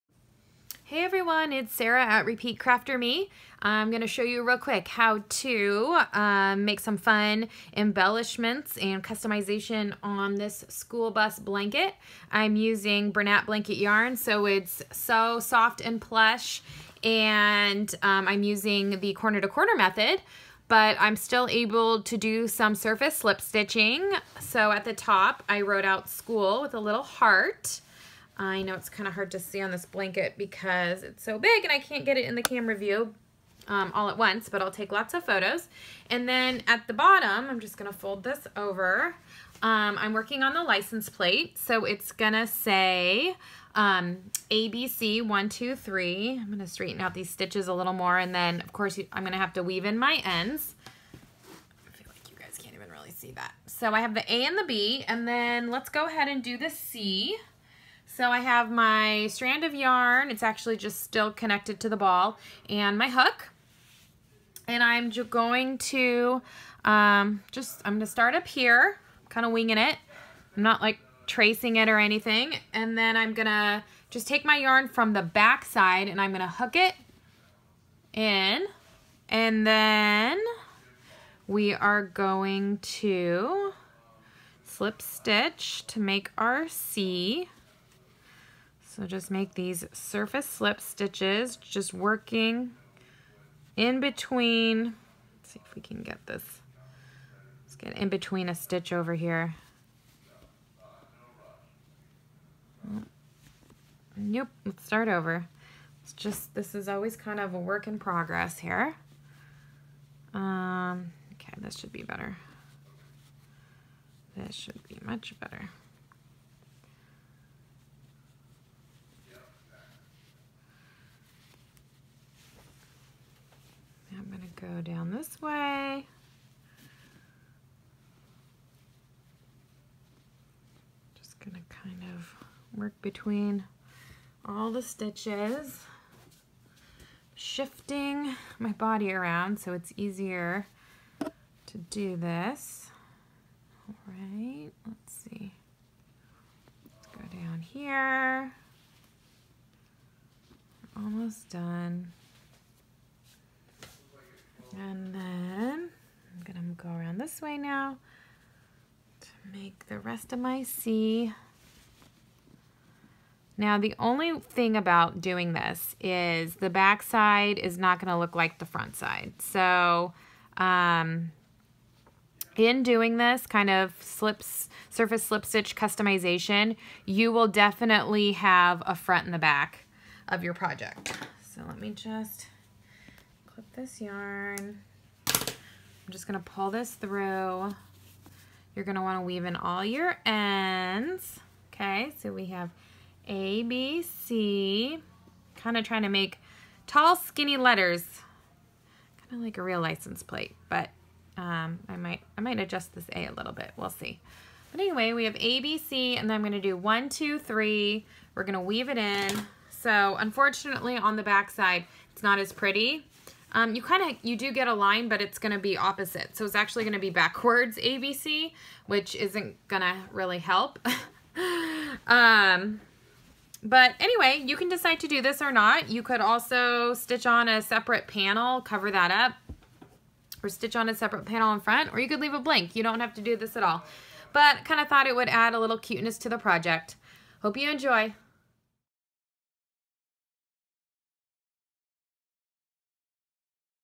School Bus Blanket: Surface Slip Stitch (Crochet)
Surface Slip Stitching on top of the School Bus Blanket! This is a corner-to-corner crochet blanket using Bernat Blanket Brights Yarn. Pattern is available on Yarnspirations.com!